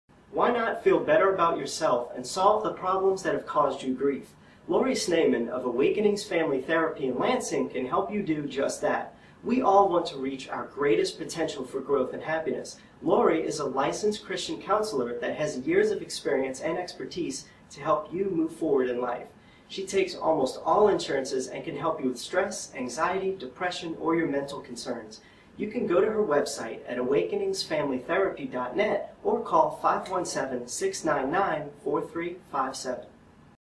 Christian Counseling Center in Holt, MI
This young man is excited the progress he made while in counseling in Lansing, MI and wants to encourage you also to get help.  Awakeningsfamilytherapy.net or lansingchristiancounselors.com